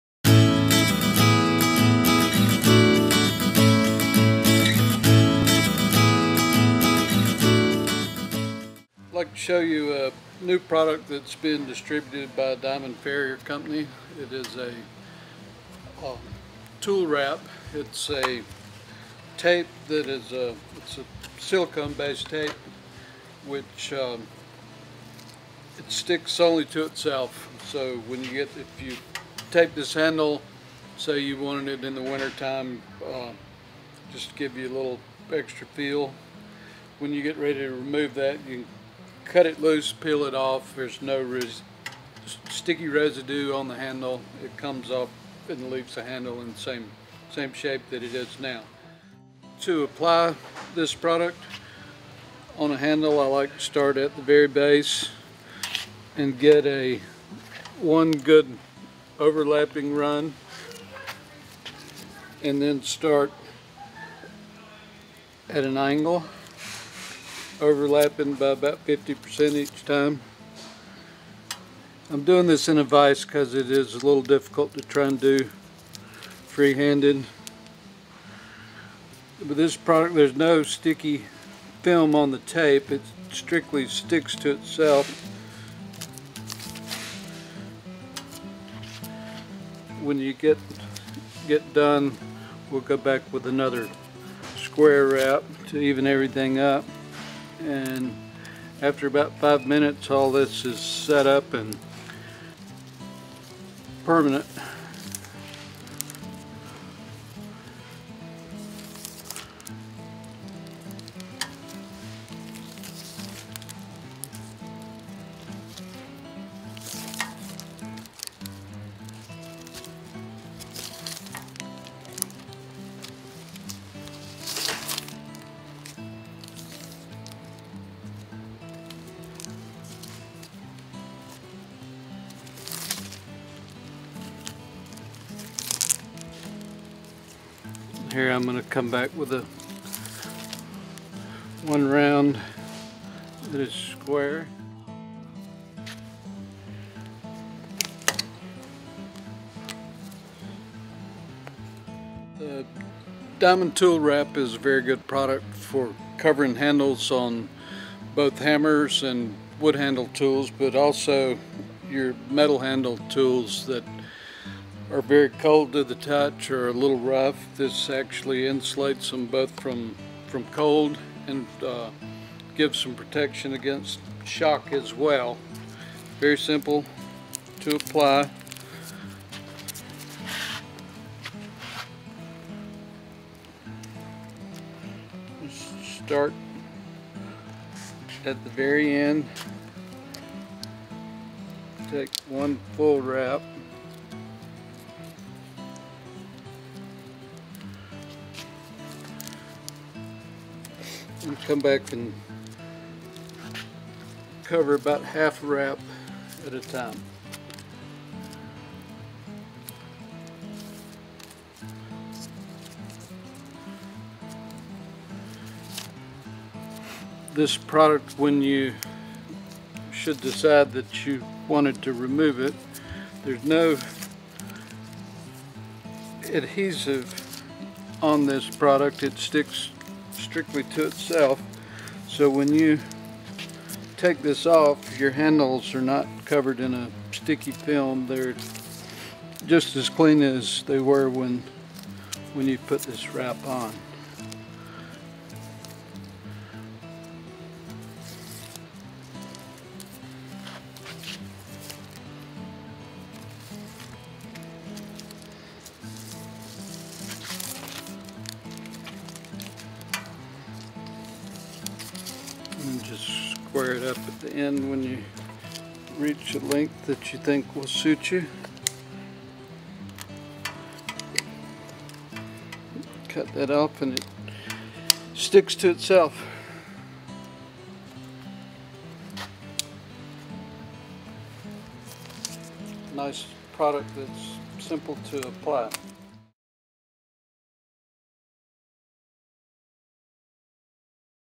FootPro Silicone Tool Handle Wrap
The FootPro Silicone Tool Handle Wrap is a versatile tape that works well as a gripping surface for tool handles. The tape will fuse onto itself after two minutes and will not allow repositioning. It has a very high tensile strength, will not melt up to 500°F, stays flexible to -60°F, and will stretch up to 300%. This tape resists UV rays and weathering.